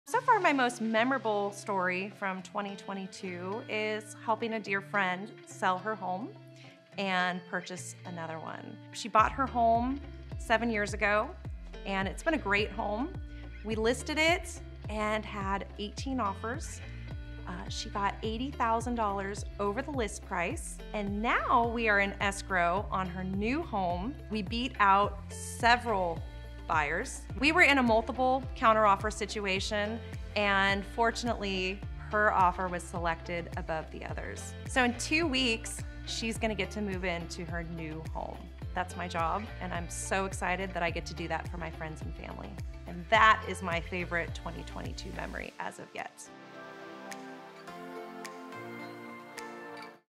My Favorite Real Estate Memory of 2022 | Julie Boyadjian | Pacific Sotheby's International Realty
So far my most memorable story from 2022 is helping a dear friend sell her home and purchase another one. She bought her home 7 years ago and it's been a great home. We listed it and had 18 offers. She got $80,000 over the list price. And now we are in escrow on her new home. We beat out several offers. We were in multiple counter-offer situations and fortunately, our offer was selected above the others. In two weeks she's going to move into her new home. That's my job and I'm so excited that I get to do that for my friends and family. That is my favorite 2022 memory thus far.